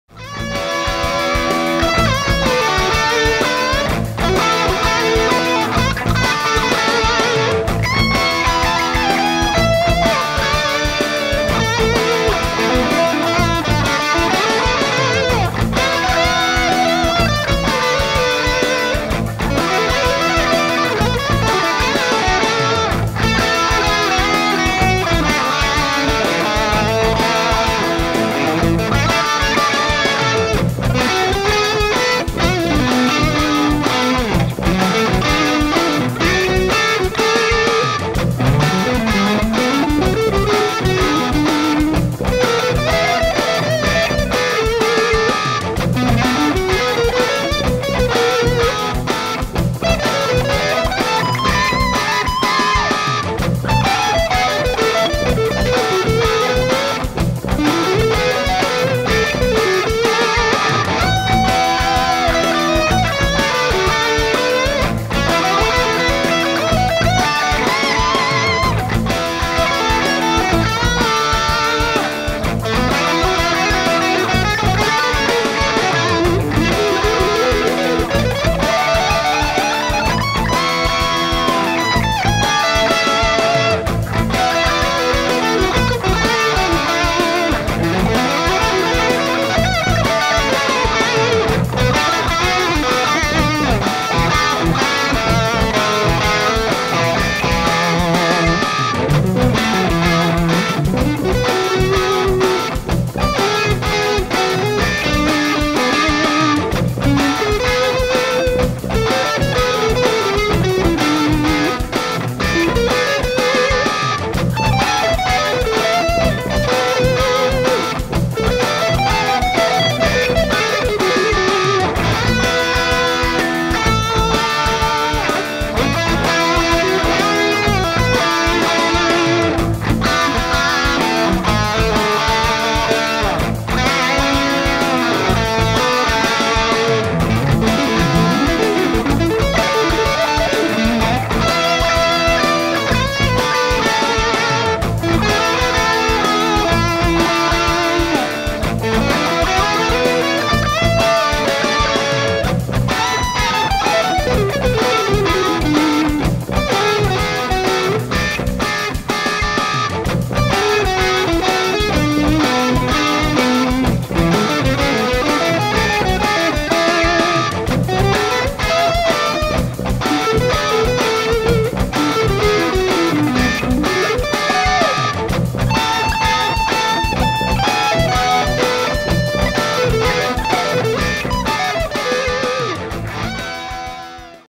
Improvise oops #4
Some improvisation over my own backing track. Expect mistakes, won`t be disappointed.